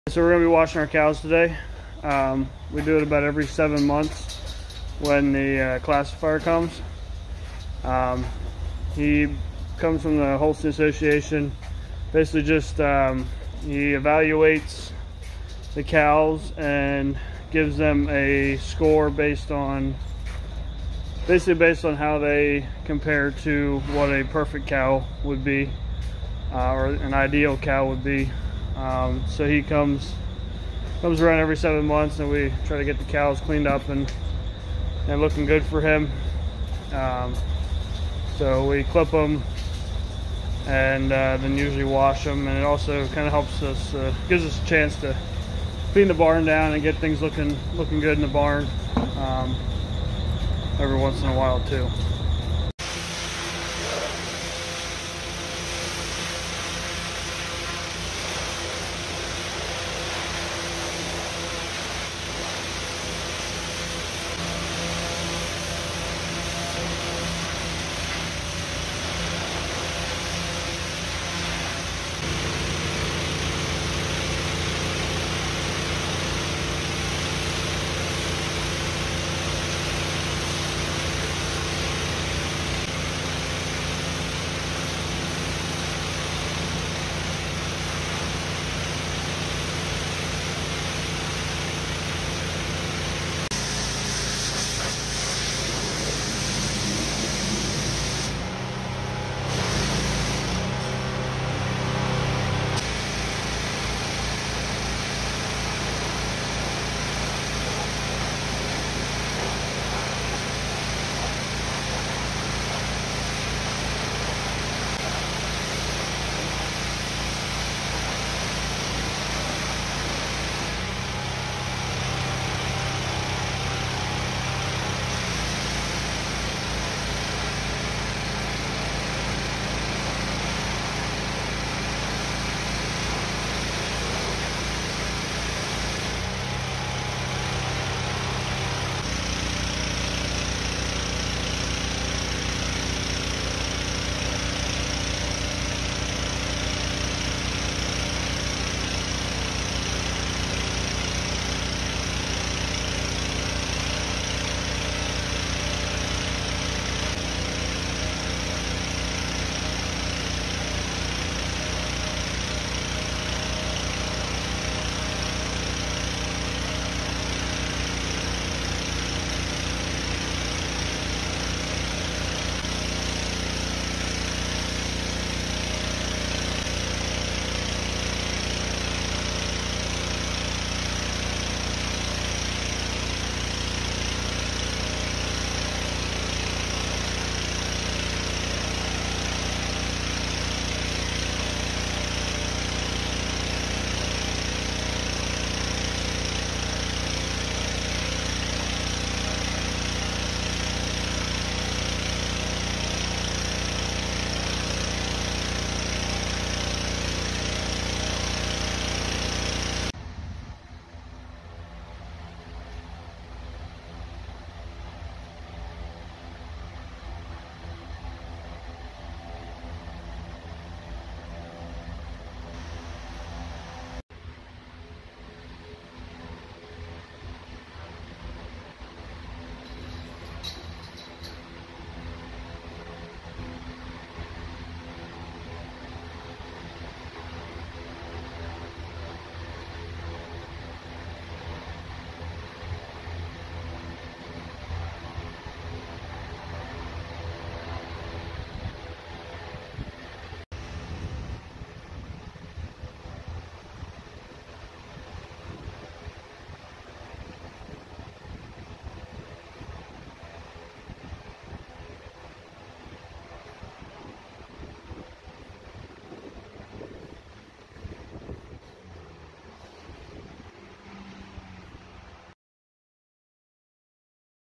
Washing cows
Power washing the girls right in the barn. Trust me, they love it!